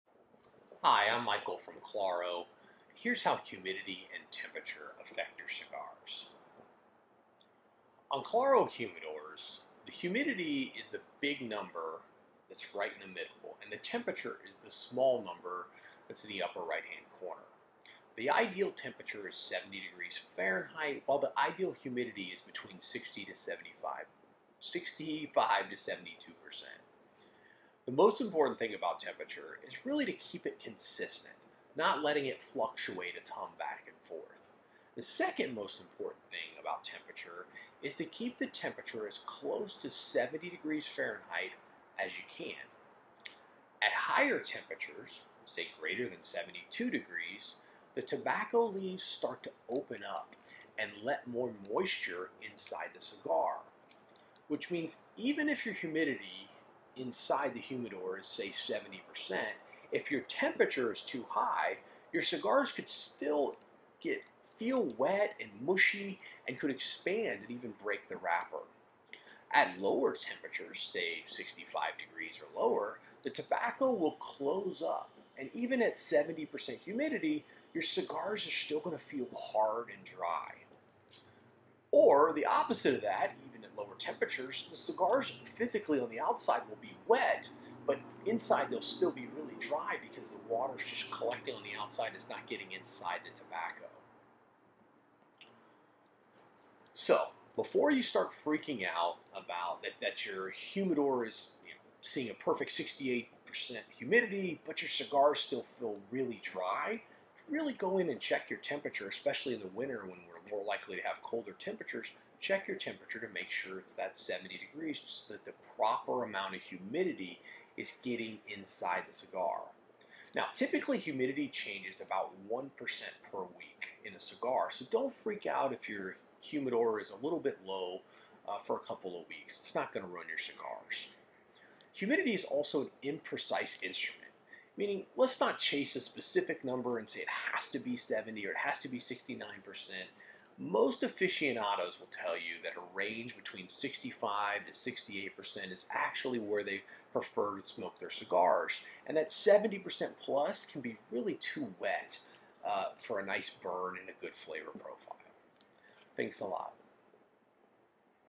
How Temperature and Humidity affect your cigars
2 minuts to become an expert on how temperature and humidity affect your cigars inside of a humidor.